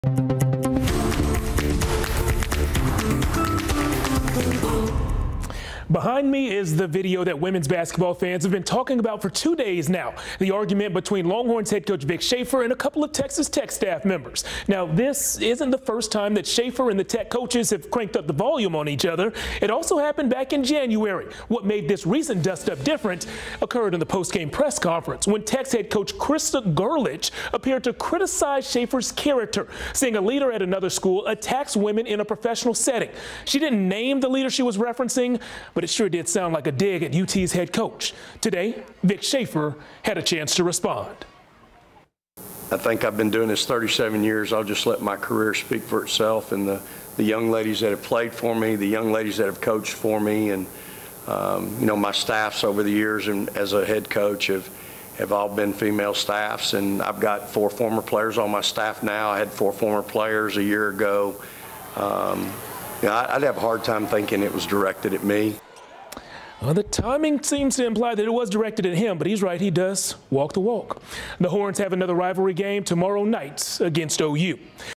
Texas WBB head coach responds to Texas Tech claims | KVUE
Tech's head coach appeared to criticize Vic Shaefer following a brief argument at the recent game.